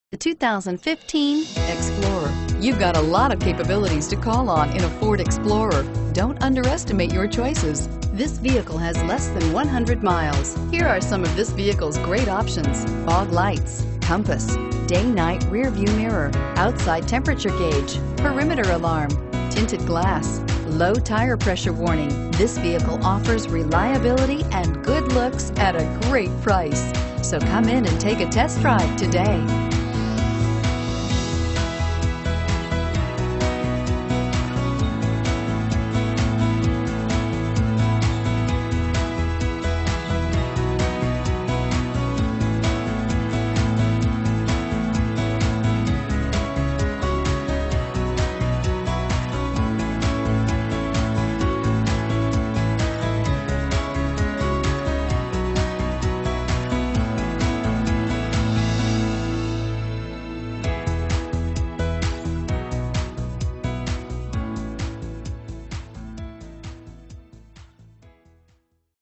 2015 Ford Explorer Greeley CO R69353
Pick up this White Platinum Metallic Tri-Coat 2015 Ford Explorer, available today at Spradley Barr Ford. Featuring Automatic transmission, it has 15 miles. This could be the one you've been searching for.